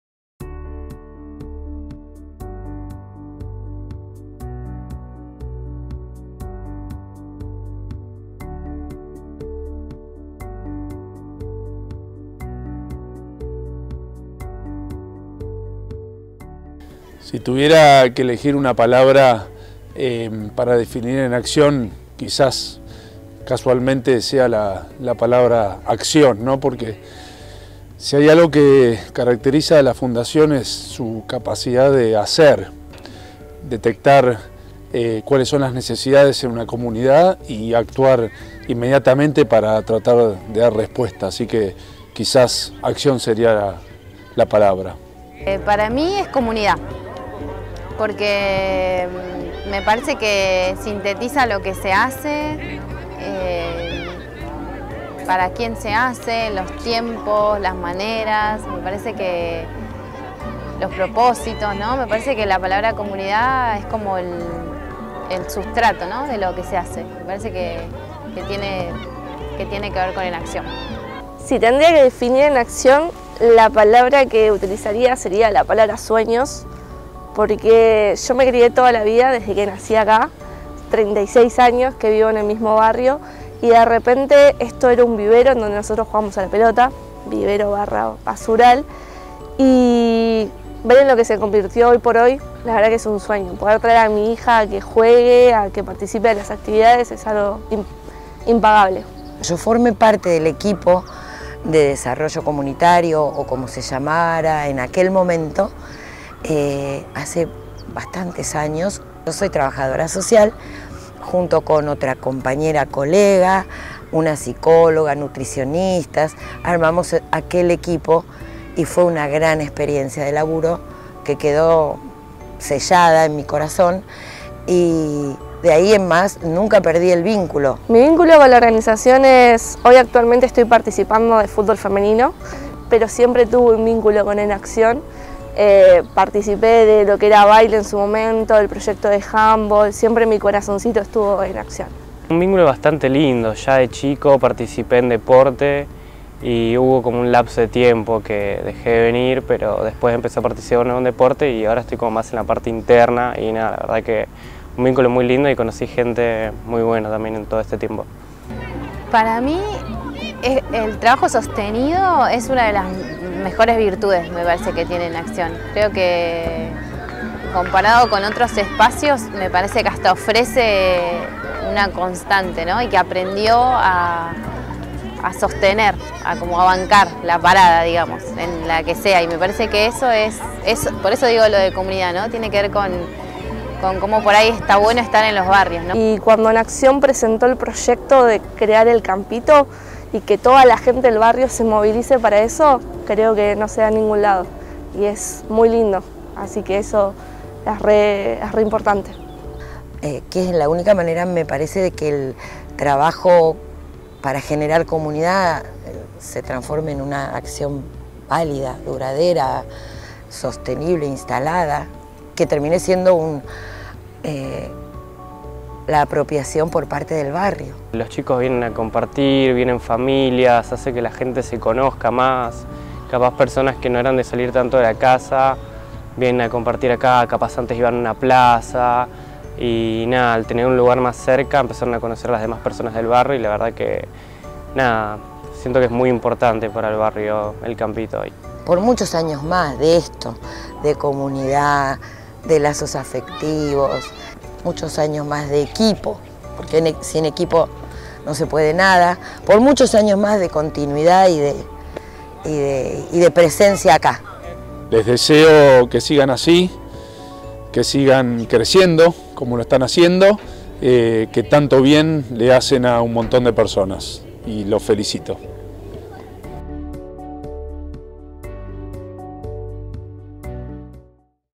25 años, 25 historias (3er episodio)
Estamos cumpliendo 25 años. Durante todo ese tiempo crecimos a la par, construimos espacios, experiencias con y en COMUNIDAD.
En este aniversario nos tomamos un momento para pensar en ¿cómo hacer para contar todo lo que sucedió en este tiempo? Y porque estamos convencidos de que las HISTORIAS, de la misma manera que una COMUNIDAD, se construyen con otros; creemos que la mejor manera de contarles estos 25 años es a partir de la voz de quienes fueron parte.
Por eso, invitamos a diferentes personas que transitaron los momentos, los lugares, los logros, los tropiezos y las alegrías de la Organización EnAcción a conversar y recordar con nosotros.

En este episodio Fernando, Luciana, Pamela, Amalia y Juani comparten sus historias en EnAcción. Son estos relatos, estas miradas y estas experiencias las que construyen y enriquecen la historia de nuestra organización.

¿Qué deseo tenés vos en estos 25 años?